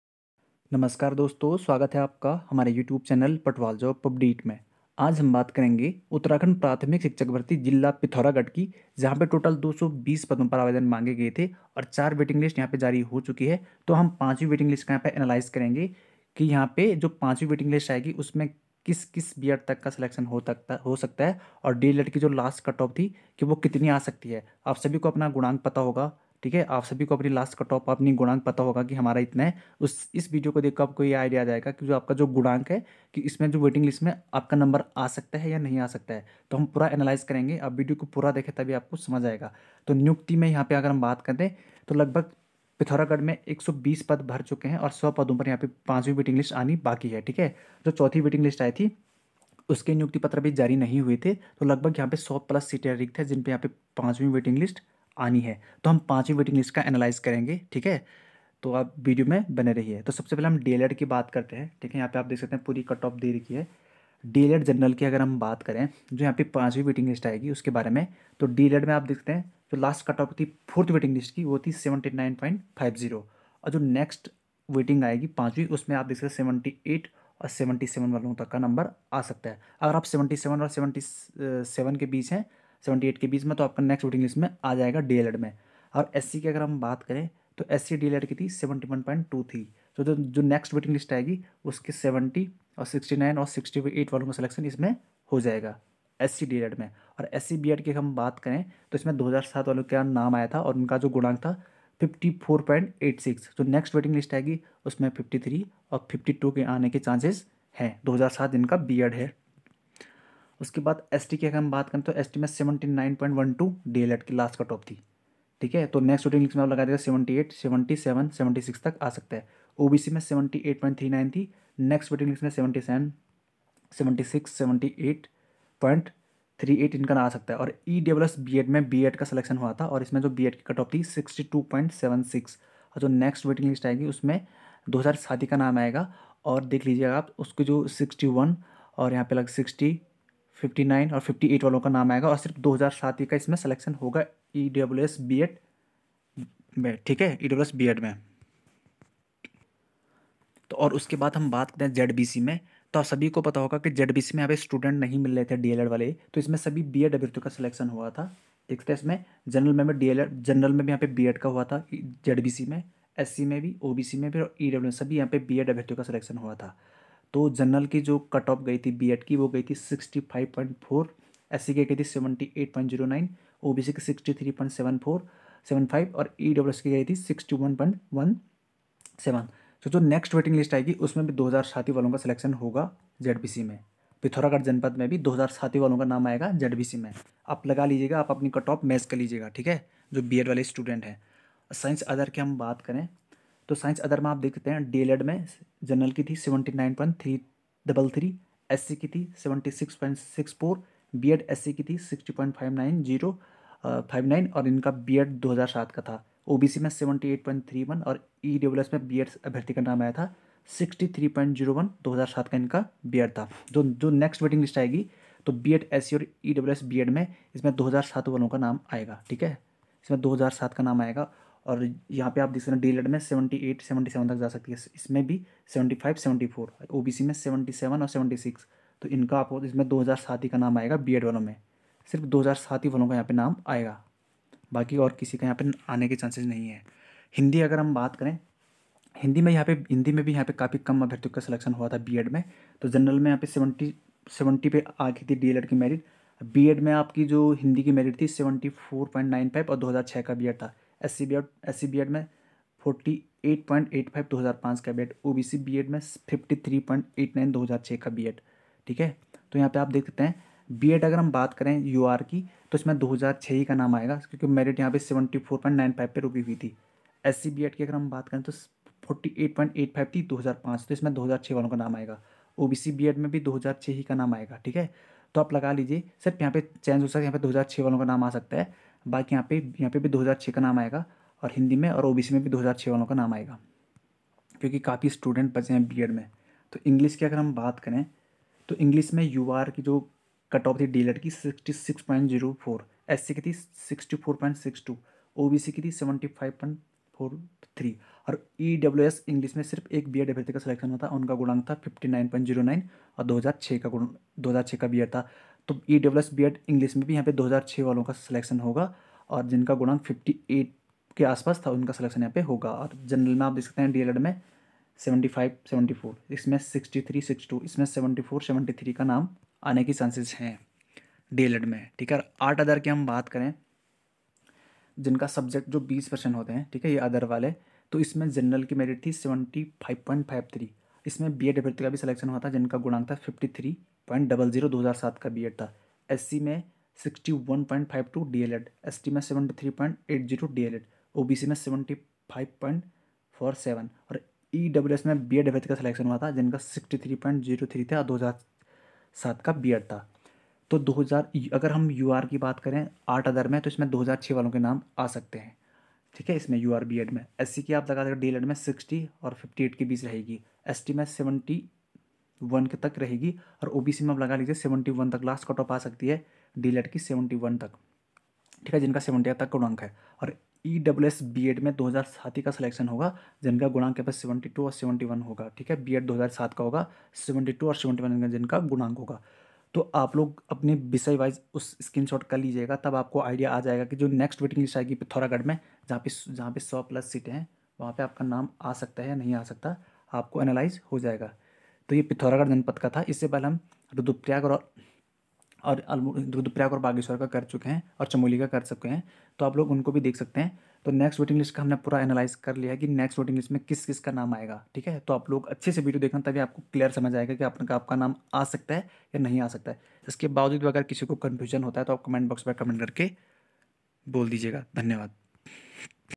प्राथमिक शिक्षक भर्ती पिथौरागढ़ 5 waiting list analyzation uttarakhand primary teacher latest news
उत्तराखंड प्राथमिक शिक्षक भर्ती जिला पिथौरागढ़ में नियुक्ति की बात रिक्त पदों का विवरण
5वी वेटिंग लिस्ट का एनालाइजेशन